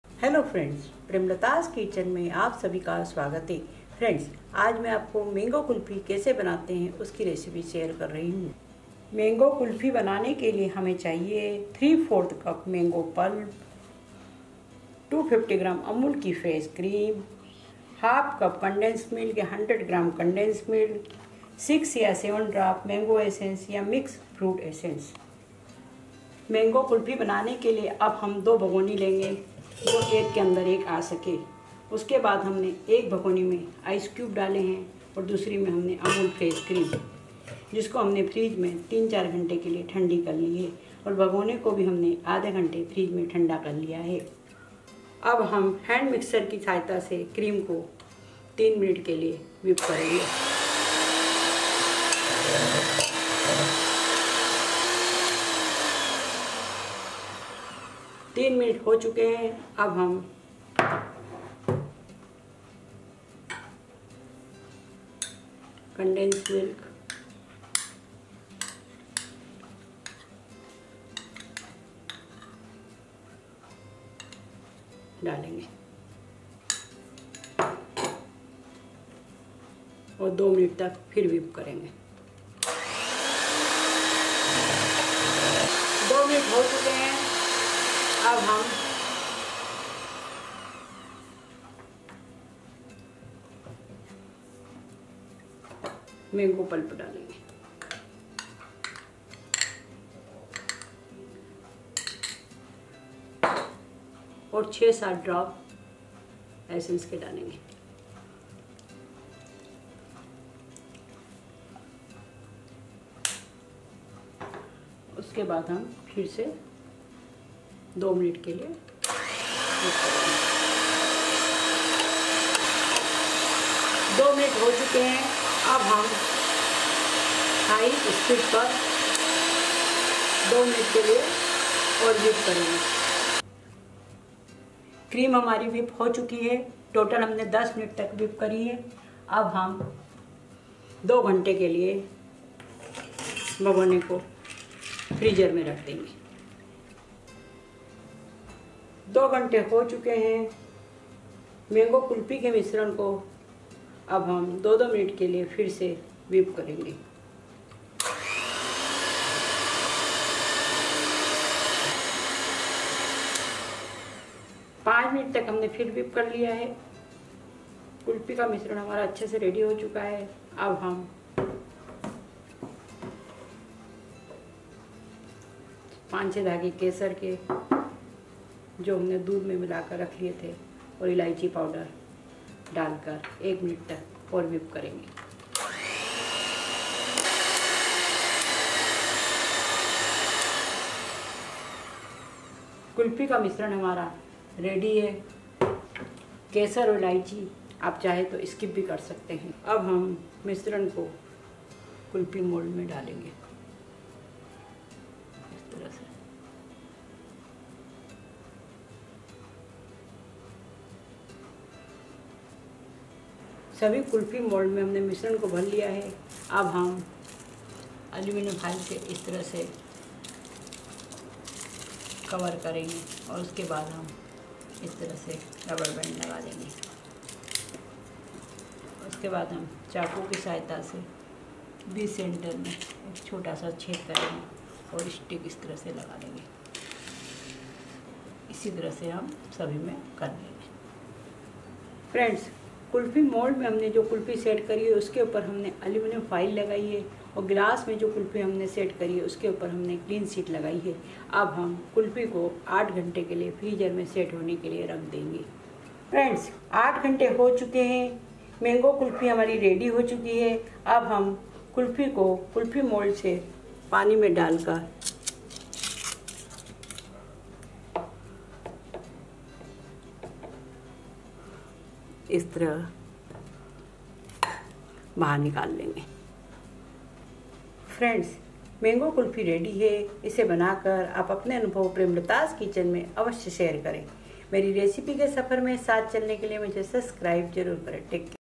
MANGO OR AAM KULFI WITH EASY INGREDIENTS NO BREAD NO CORNFLOUR
MANGO OR AAM KULFI WITH HOME INGREDIENTS NO BREAD NO CORNFLOUR SWEET RECIPE IN HINDI BY PREMLATA'S KITCHEN EPISODE 222